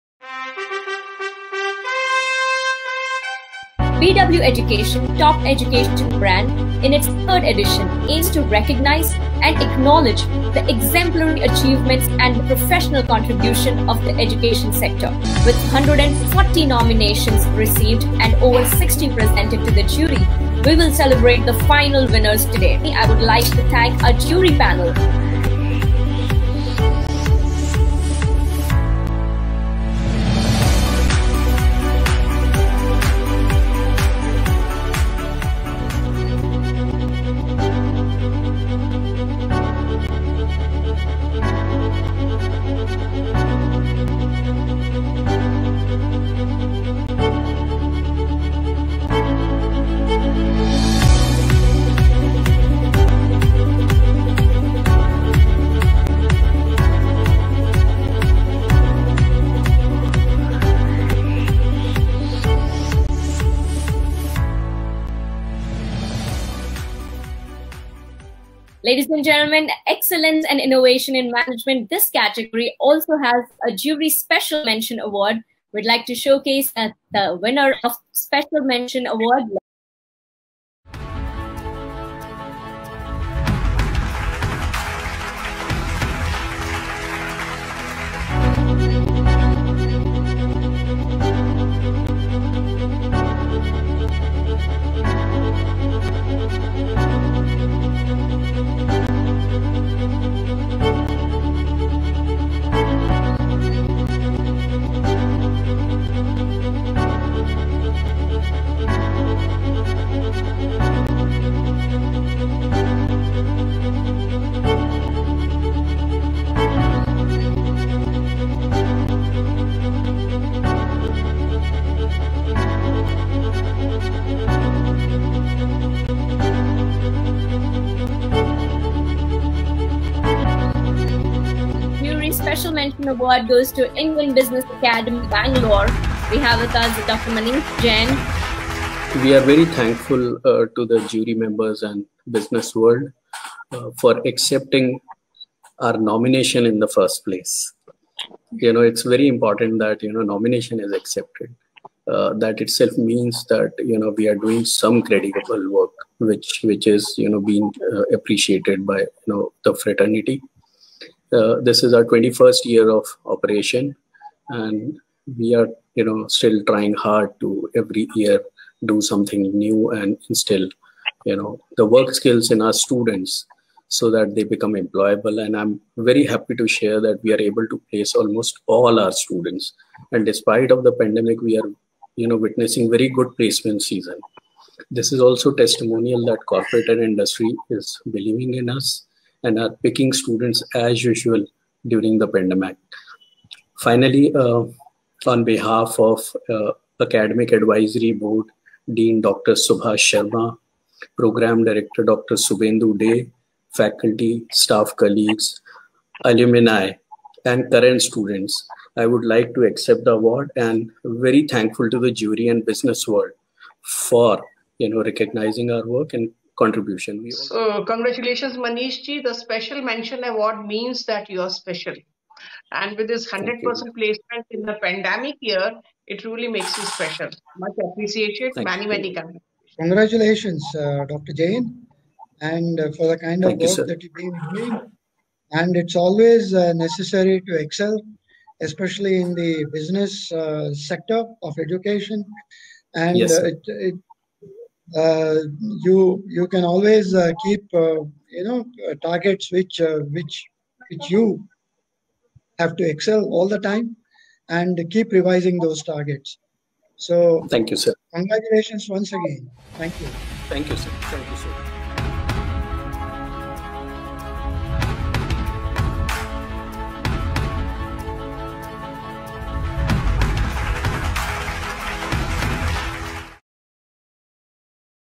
IBA Bangalore - Top Education Brands Award (Management) 2020
Indus Business Academy (IBA), Bengaluru is one of the top MBA colleges in Bengaluru & receive an International Accreditation by IACBE.

IBA (Indus Business Academy, Bangalore is chosen as one among the Top Education Brands (under Category - Excellence and Innovation in Management) 2020 and got 'Special Mention' by the Jury.